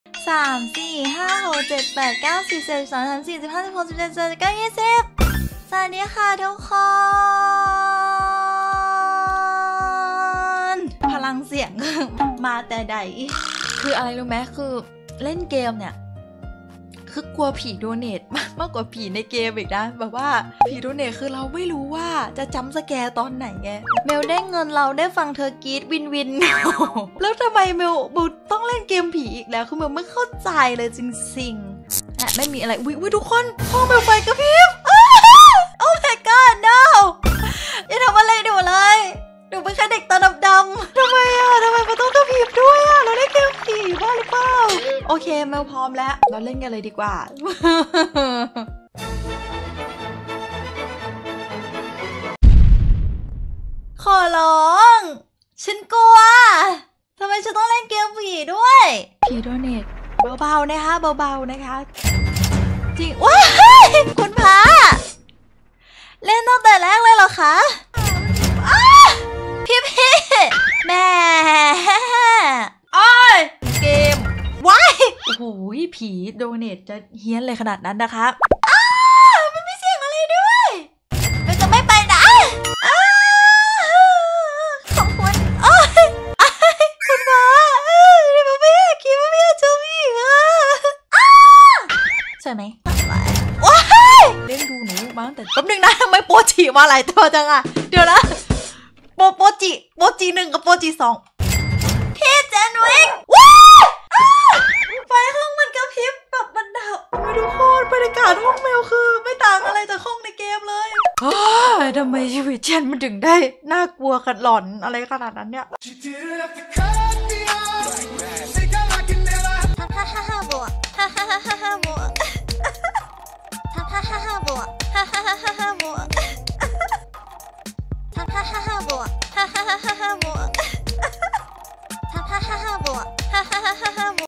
[Vtuber]Hi-light ไฮไลท์นี้ มีแต่กรี๊ดโดเนท!
แทบไม่มีผี ไม่มีอะไรน่ากลัวเลย ยกเว้นไฟในห้อง.. @MycaraMelony